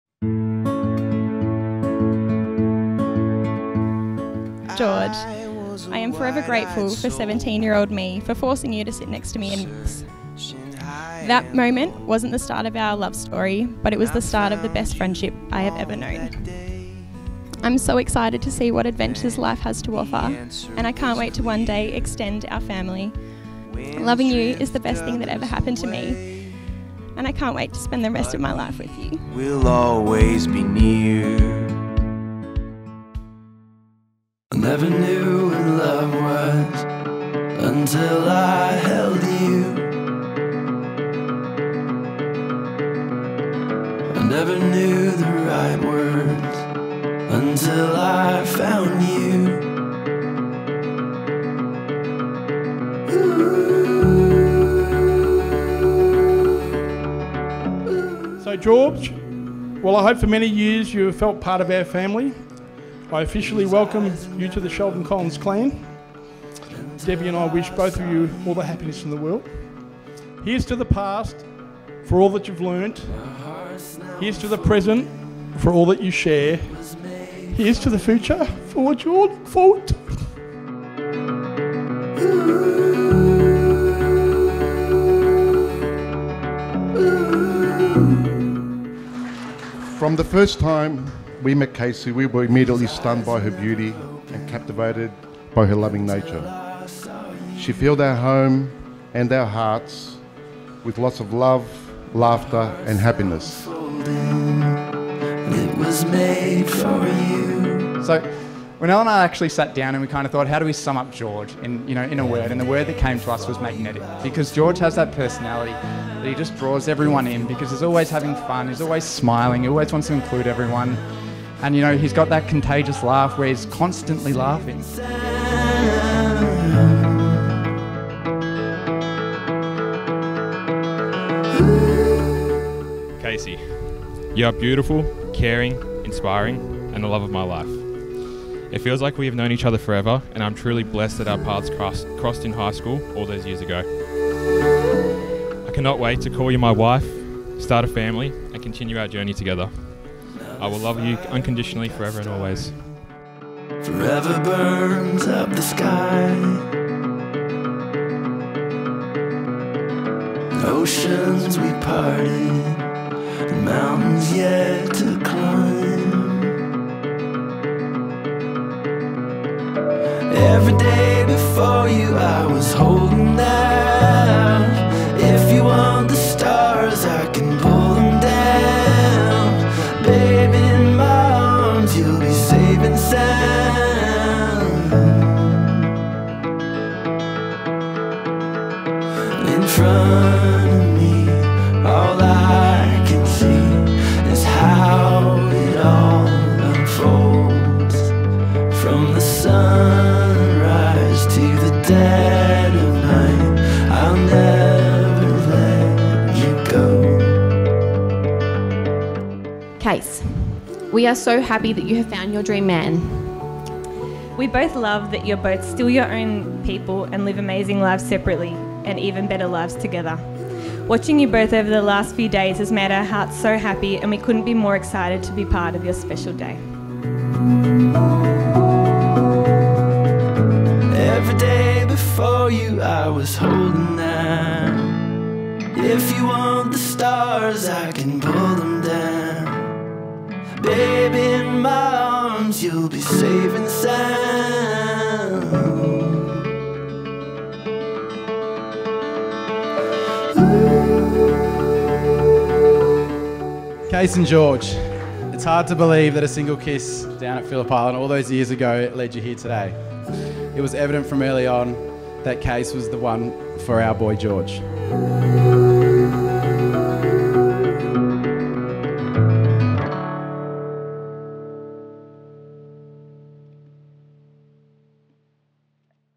Casey and George - Wandin Park Estate
The beautiful wedding of Casey and George - Wandin Park Estate in Melbourne, Australia.

Filmed and edited by Momento Films.